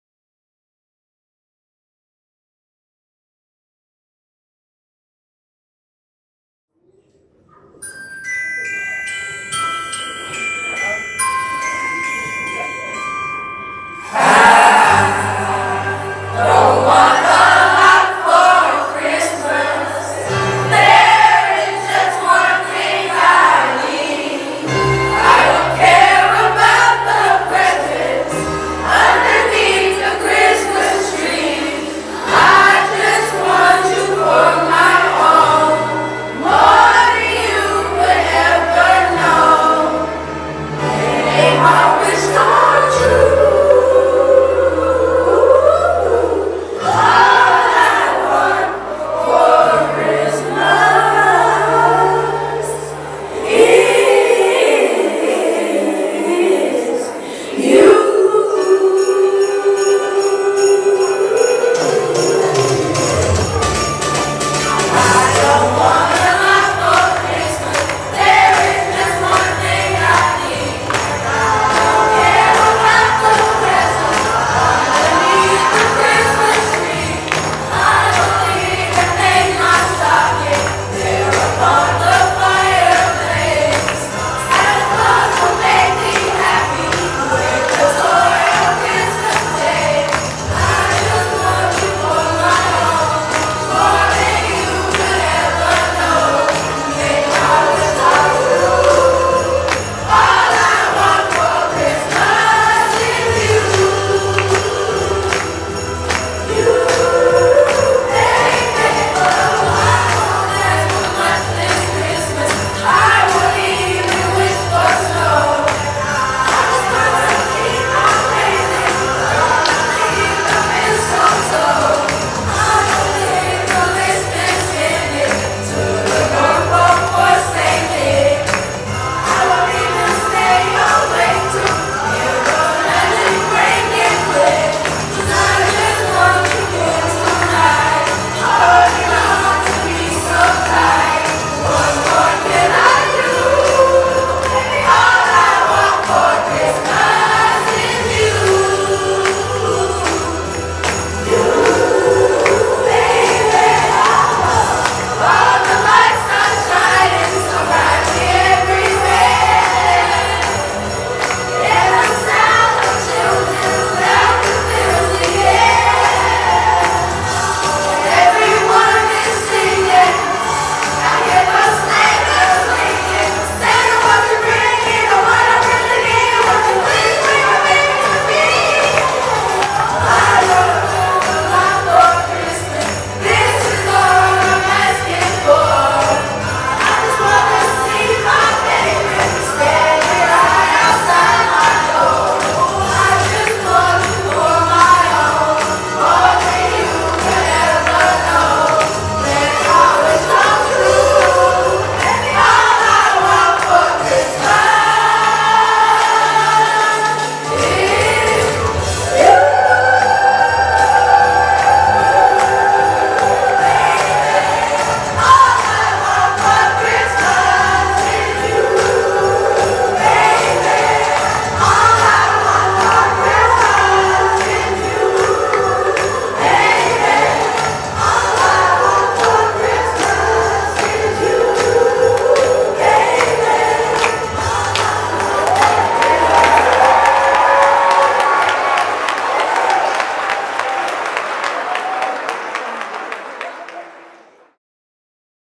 St Ailbe Christmas 6th 7th
🎄✨ Enjoy the festive spirit with St. Ailbe Catholic School's heartwarming Christmas program! Watch as our talented students spread joy and cheer through enchanting songs and performances that capture the magic of the holiday season. From classic carols to spirited renditions, their voices will warm your heart and fill you with the joy of Christmas. Join us in celebrating the spirit of togetherness and the joy of giving as our students share their talents and spread love through music. 🎶🌟